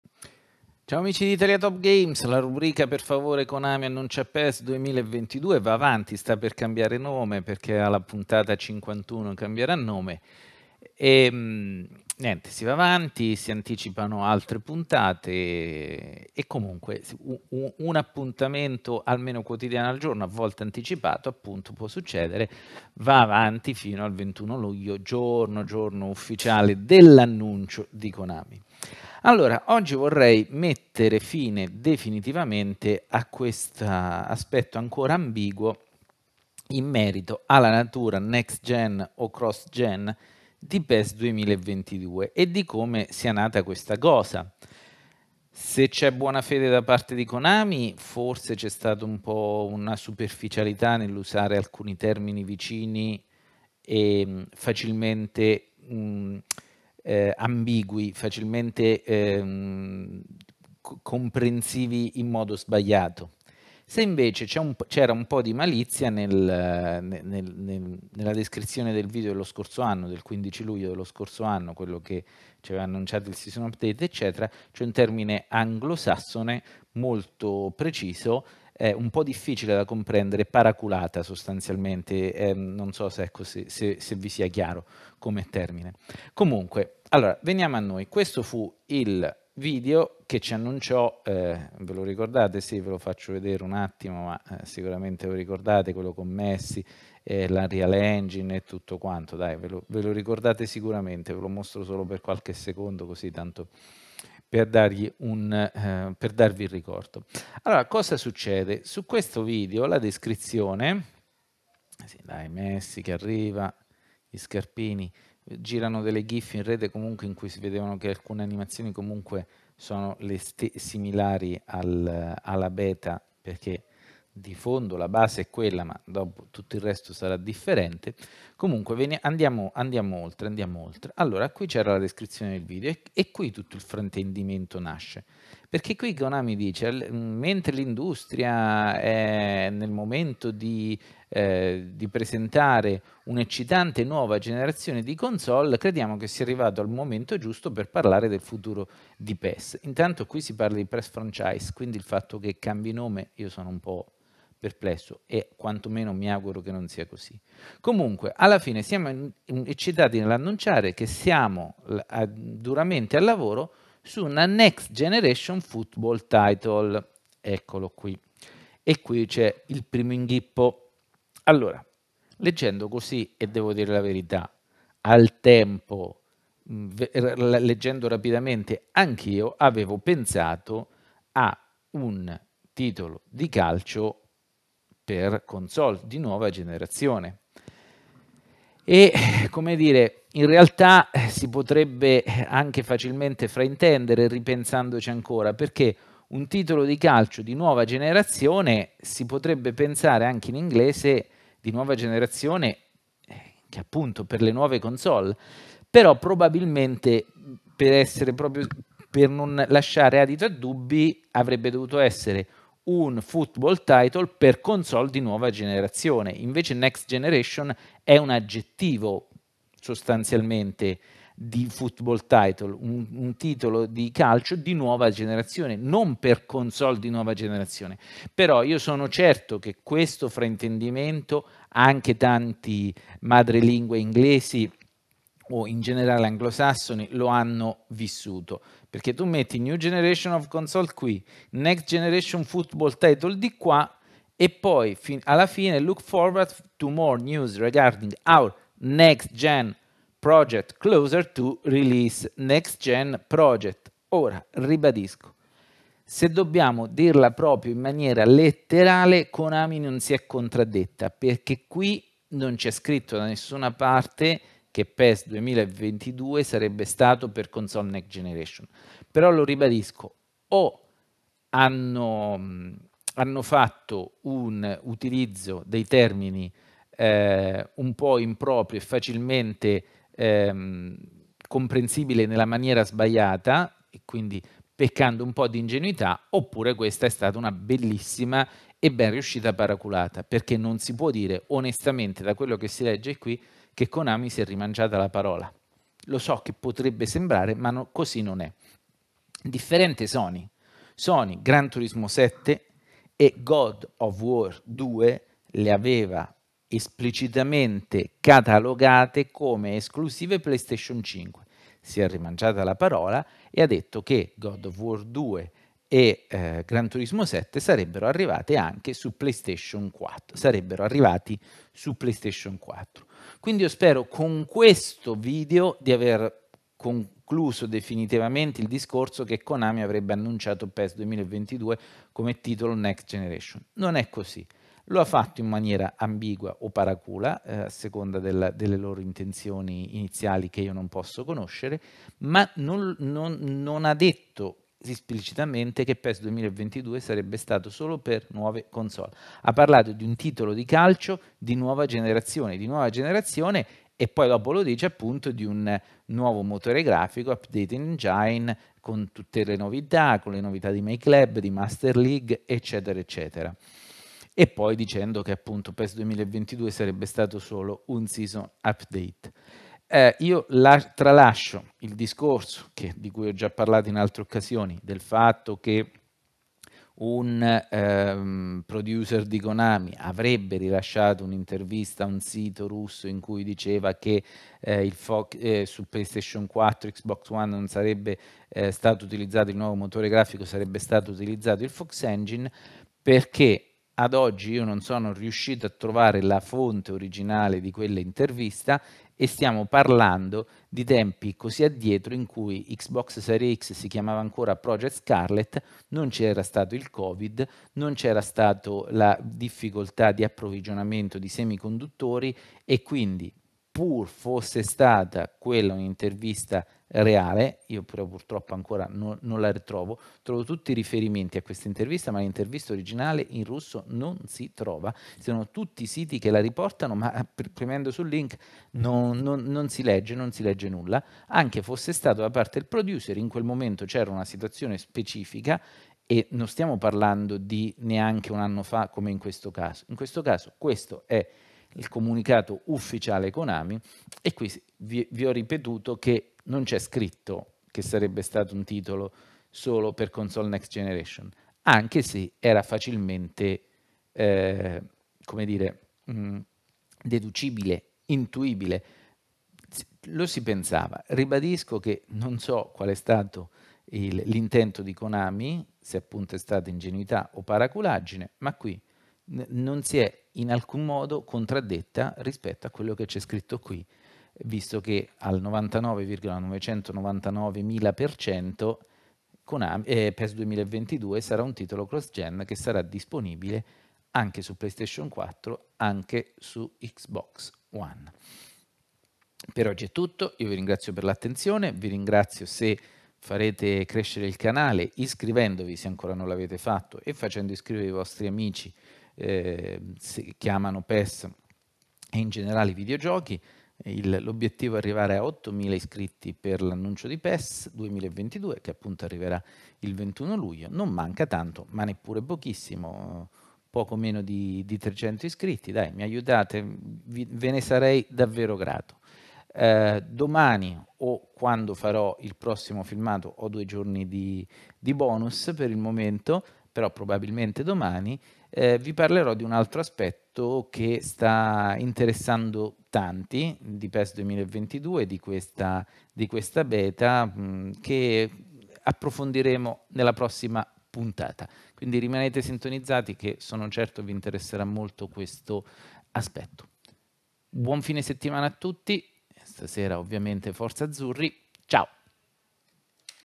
PES 2022 fu "annunciato" solo Next Gen da Konami? Facciamo chiarezza DEFINITIVAMENTE - PFKAP22 G50A
PES 2022 fu annunciato solo Next Gen da Konami il 15 luglio dello scorso anno con il primissimo video che aveva come protagonista Lionel Messi? Mi auguro che con la puntata di oggi di "Per Favore Konami annuncia PES2022" sia chiaro a tutti come sono andate davvero le cose.